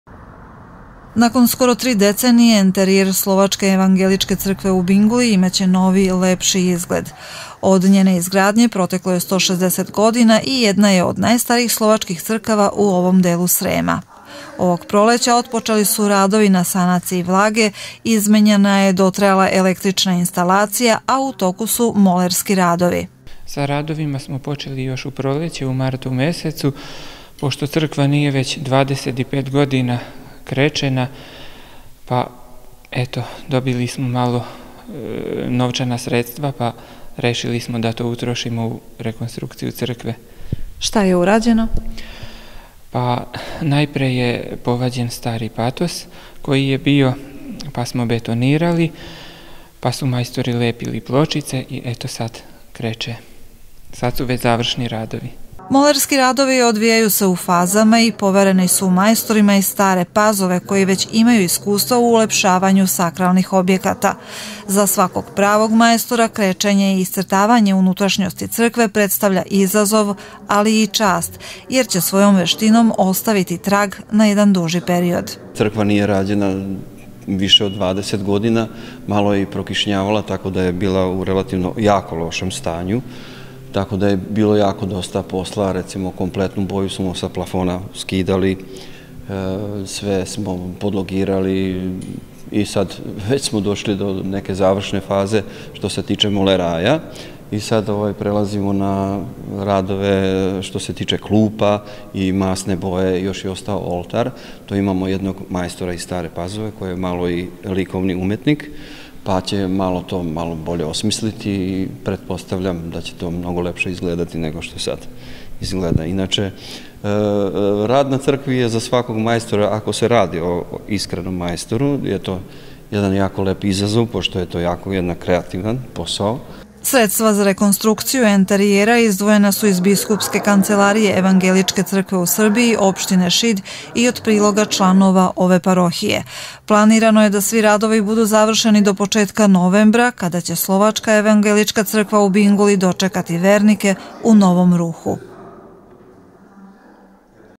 Rekonstrukcija enterijera crkve u Binguli
U Slovačkoj evangeličkoj crkvi u Binguli privode se kraju radovi rekonstrukcije enterijera ovog sakralnog objekta. Početkom proleća urađeni su radovi na izmeni starog patosa, saniranju vlage u zidovima i dotrajale elektro instalacije. Trenuto su u toku molerski radovi koji će do kraja meseca biti završeni.